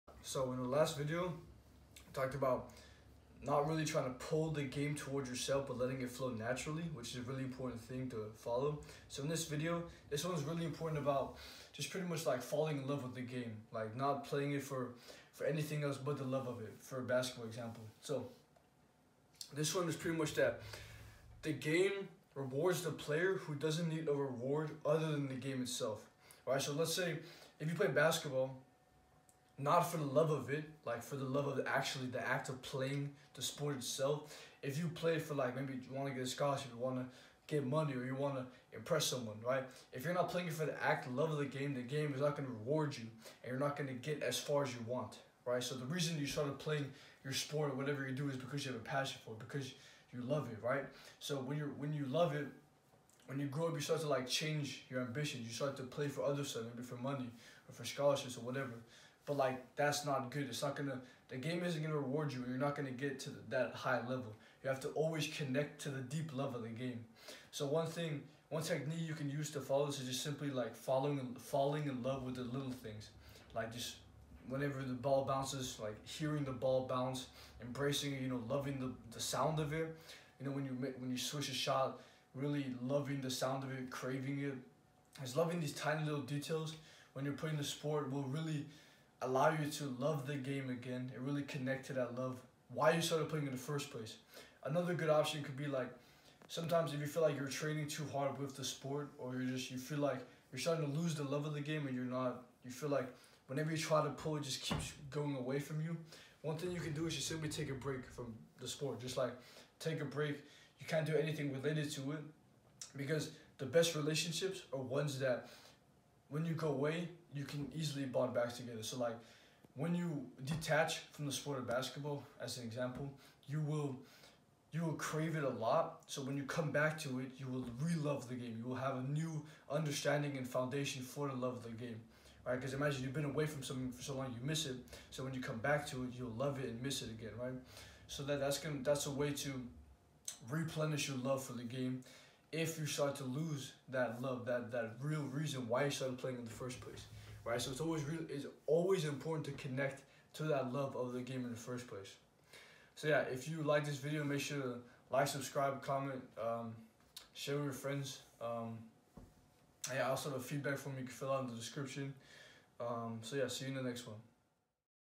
Reconnect to the love of the game!
In this video, I will be talking about connecting. to the reason you started playing basketball in the first place.